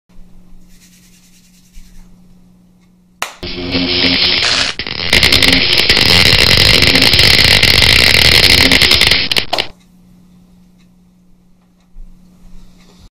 I'm starting to get the hang of using "Force lightening"
Just started learning a few tricks with HitFilm Express.